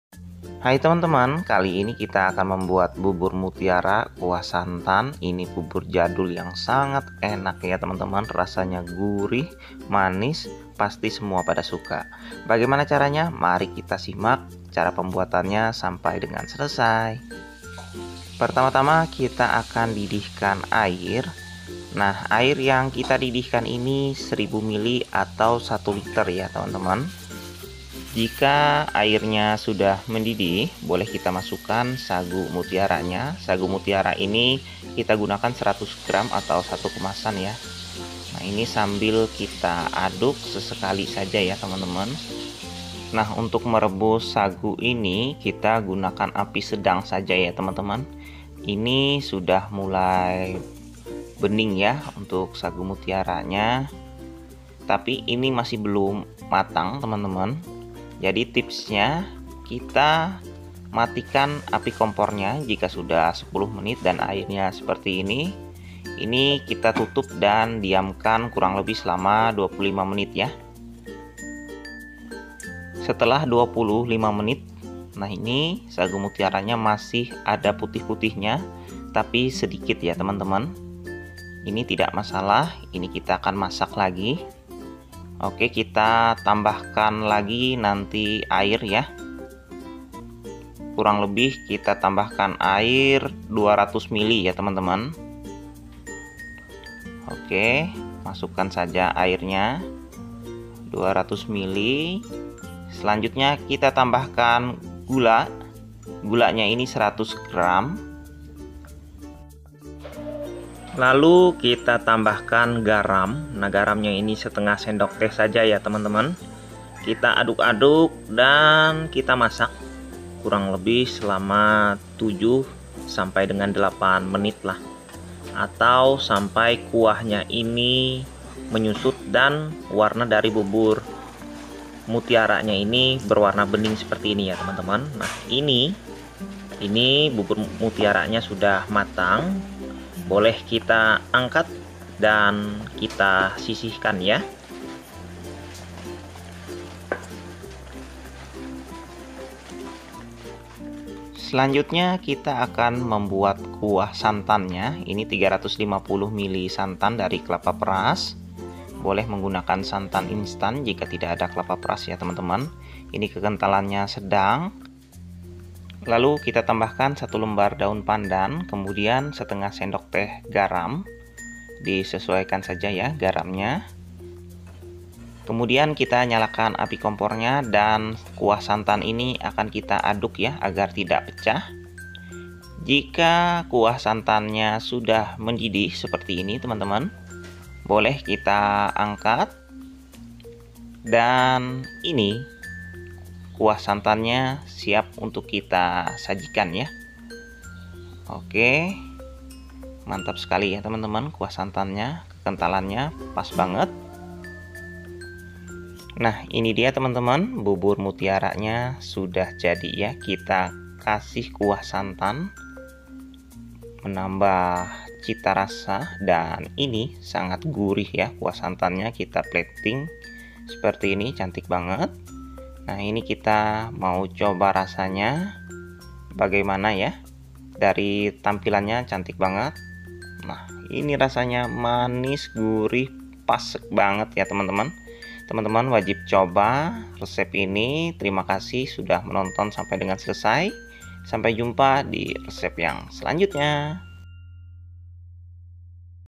Resep bubur muatiara kuah santan jadul yang sangat enak dan gurih
Resep Bubur Mutiara Kuah Santan Jadul Sangat Gurih Dan Enak
Bahan:
100 gr sagu mutiara (satu kemasan)
1000 ml air + 200 ml air
100 gr gula
1/2 sdt garam

Bahan kuah santan:
350 ml santan perasan kelapa (jika tidak ada boleh diganti santan instan 200 ml + 150 ml air)
1 lembar daun pandan
1/2 sdt garam